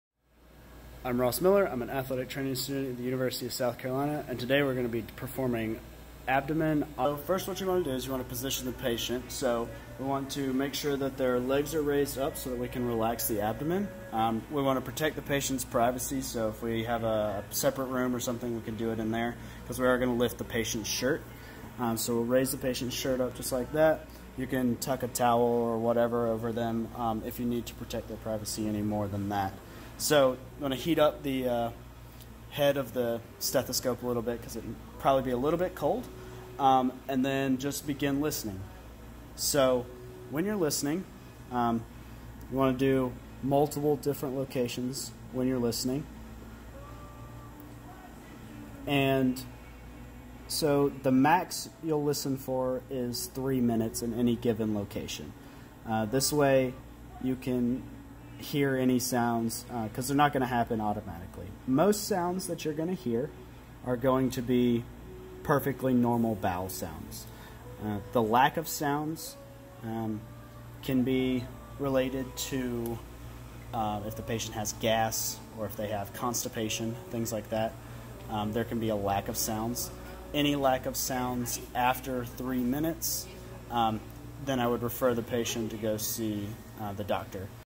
Abdomen Auscultation
Ross Miller, third year AT student, preforms abdomen auscultation